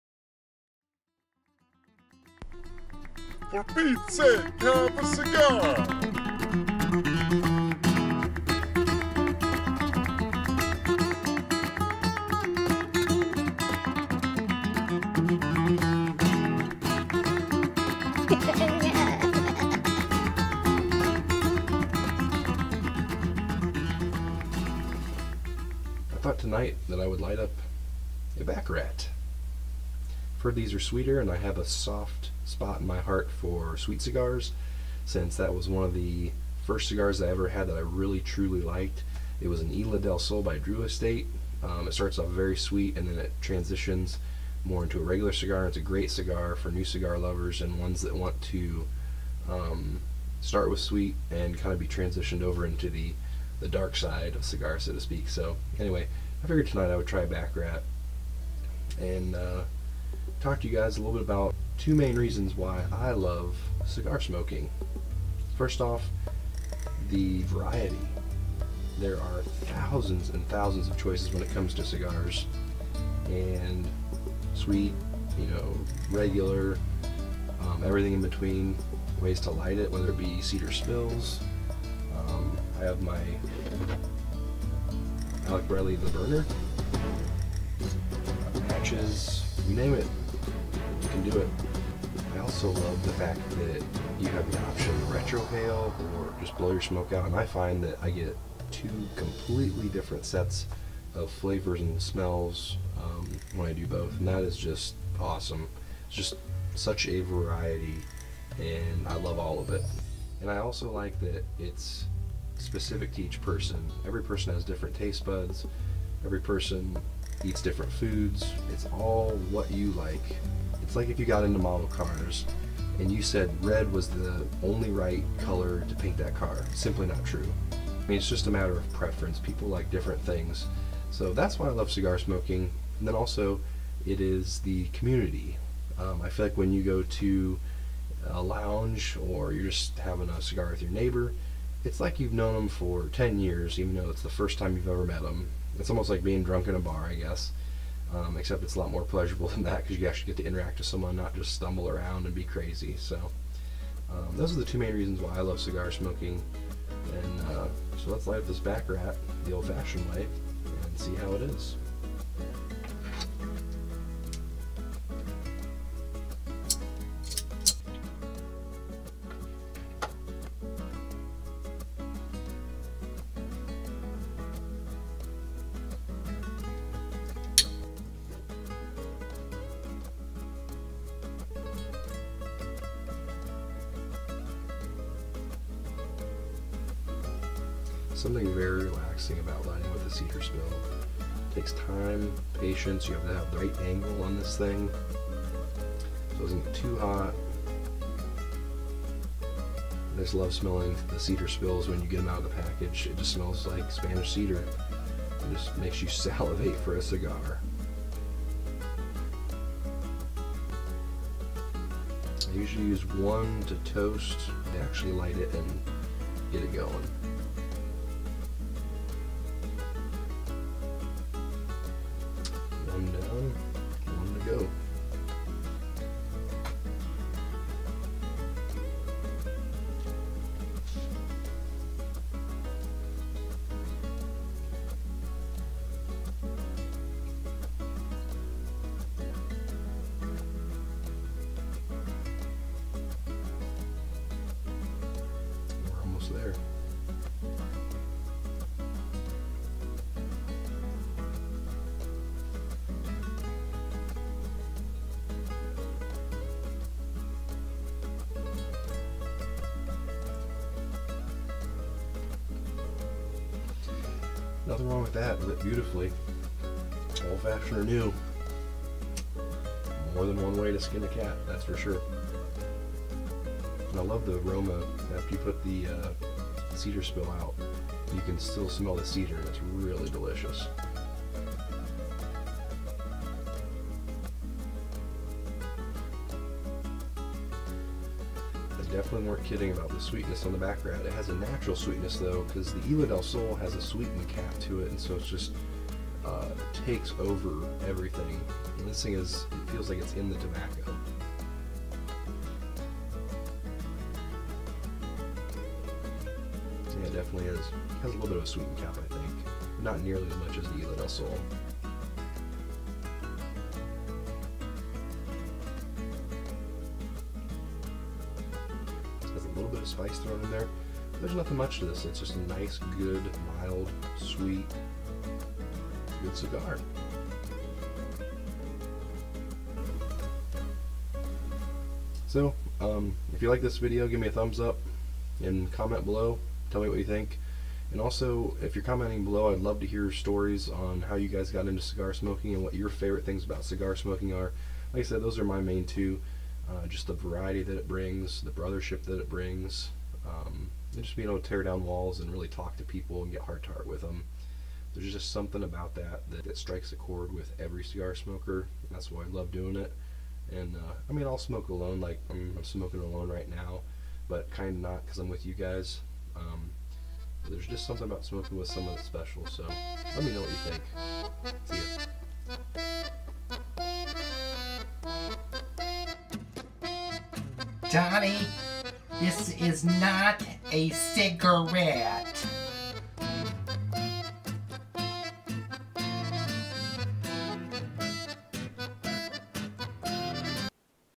WHY I LOVE CIGARS!!!
Made this clip while having a baccarat. I just love everything about cigars. They are fun and connect people in a way you couldn't otherwise do without having a cigar. Hope you enjoy!😃